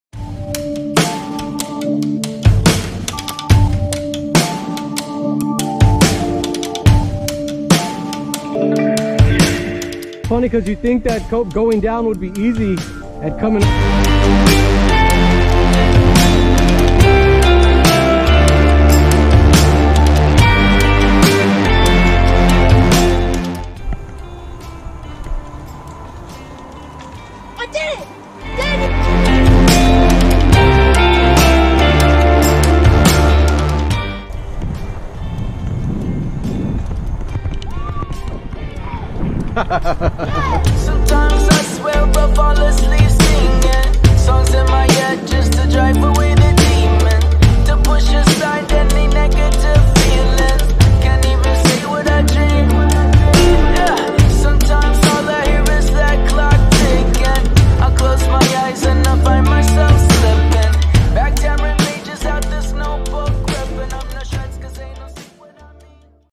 Thursday Leon creek
Took Josiah out to Leon Creek on a Friday evening ride.